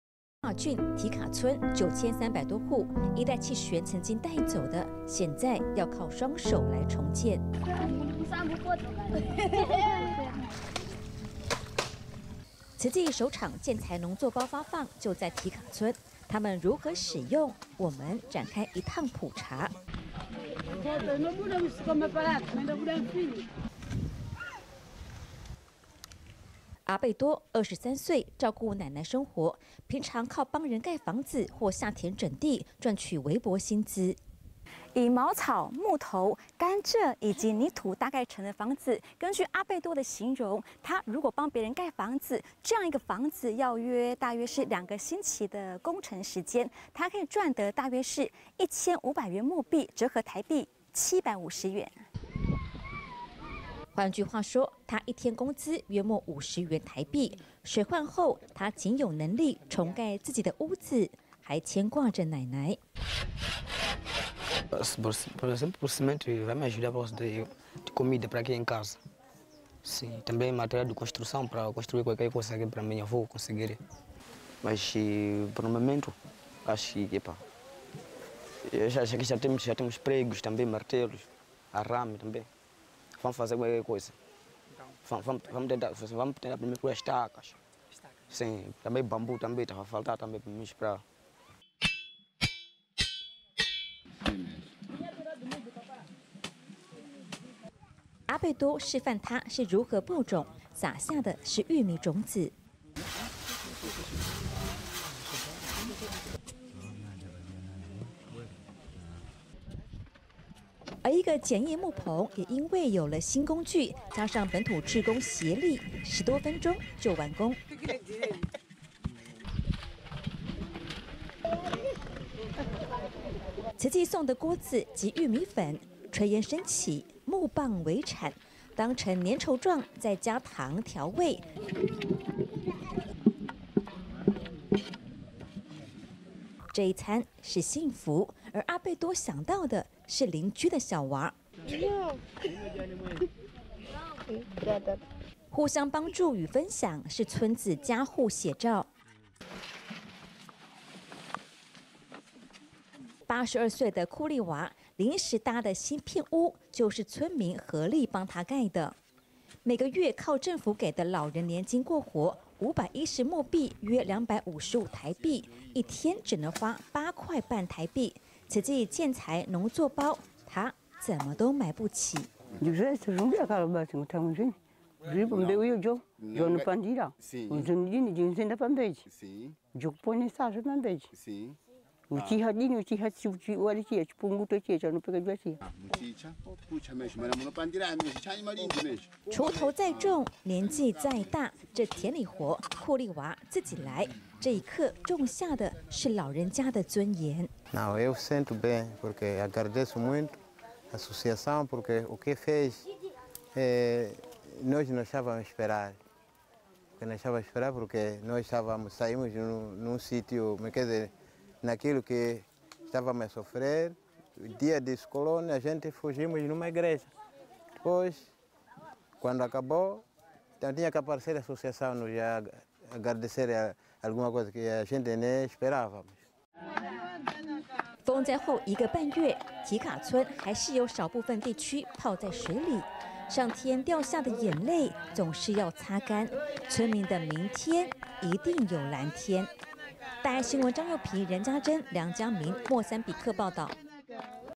再度走訪雅瑪郡堤卡村
一個月前的伊代氣旋，造成莫三比克災情嚴重，慈濟志工持續關懷。大愛記者與本土志工再度走訪災區，雅瑪郡堤卡村，了解慈濟發送的建材農作包的使用情形。

雅瑪郡堤卡村，9300多戶。伊代氣旋曾經帶走的，現在，要靠雙手來重建。

慈濟首場建材農作包發放，就在堤卡村。他們如何使用，我們展開一趟普查。

阿貝多，23歲，照顧奶奶生活，平常靠幫人蓋房子或下田整地，賺取微薄薪資。

以茅草、木頭、甘蔗，以及泥土，搭蓋而成的房子，根據阿貝多的形容，他如果幫別人蓋房子，這樣一個房子要約兩個星期工程時間，他可以賺得大約是1500莫幣，折合台幣750元。

換句話說，他一天工資約莫50元台幣。水患後，他僅有能力重蓋自己的屋子，還牽掛著奶奶。

堤卡村民 阿貝多：「現在沒有工作了，給了種子可以下田餵飽全家，給了建材工具很好，奶奶的房子需要重建，有你們給的足夠了，要蓋房子，這裡有木頭，我可以自己找，也可以跟鄰居要，最重要的，是慈濟給了這些東西與工具，是我最需要的。」

阿貝多示範他是如何播種，灑下的是玉米種子。而一個簡易木棚，也因為有了新工具，加上本土志工協力，十多分鐘就完工。

慈濟送的鍋子及玉米粉，炊煙升起，木棒為鏟，當成黏稠狀，再加糖調味，這一餐，是幸福。而阿貝多想到的，是鄰居的小娃。互相幫助與分享，是村子家戶寫照。

82歲的庫莉娃，臨時搭的鋅片屋，就是村民合力幫她蓋的。每個月靠政府給的老人年金過活，510莫幣，約255台幣，一天只能花8塊半台幣，慈濟建材農作包，她怎麼都買不起。

堤卡村民 庫莉娃：「全部東西都很需要，種子最重要，種了不會死，可以持續有收成，在收成後又有種子可以再播種，這種子對我來說很重要，真的很謝謝。」

鋤頭再重，年紀再大，這田裡活，庫莉娃自己來。這一刻，種下的，是老人家的尊嚴。

堤卡村第九區酋長 瑞卡多：「非常感恩你們(慈濟)，因為伊代氣旋，村莊什麼都沒有了，大家腦海一片空白，不知如何再生活下去，當你們(慈濟)來了 給這些物資，可以重建房子，因為農田耕種全毀了，現在有種子能播種，是你們(慈濟)給了我們新的生命，真的特別感謝。」

風災後一個半月，堤卡村還是有少部分地區泡在水裡。上天掉下的眼淚，總是要擦乾。村民的明天，一定有藍天。

張佑平 任家珍 梁家銘 莫三比克報導